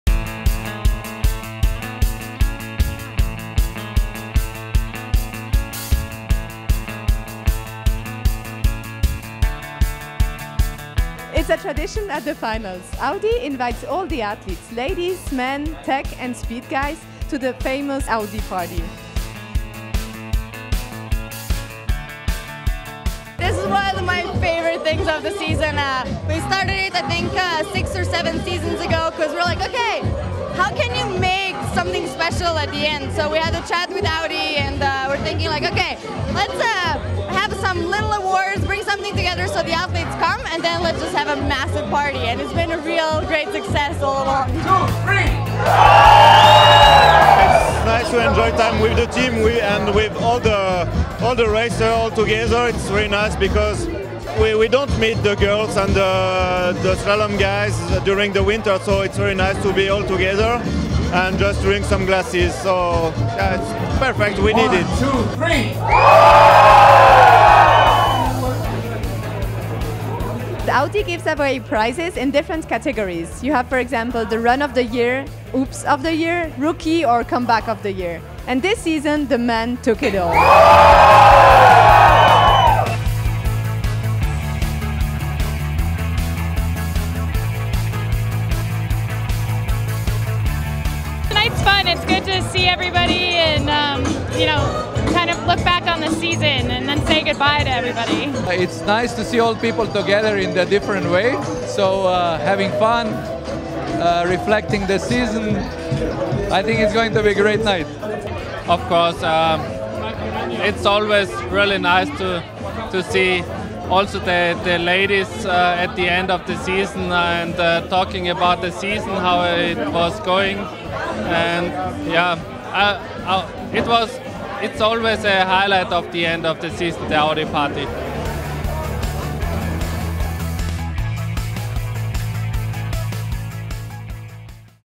Audi Quattro Night, the highlight of the end of the season - FIS Alpine
Traditionally, at the Audi FIS Ski World Cup Finals, title sponsor Audi invites athletes and teams for an "end of the season party" and hosts an unconventional award ceremony.

This year, the men took it all, and the winners are:
Comeback of the year: Beat Feuz
Breakthrough of the year: Aleksander Aamodt Kilde
Run of the year: Adrien Théaux
Ooops moment(s!) of the year: Marcel Hirscher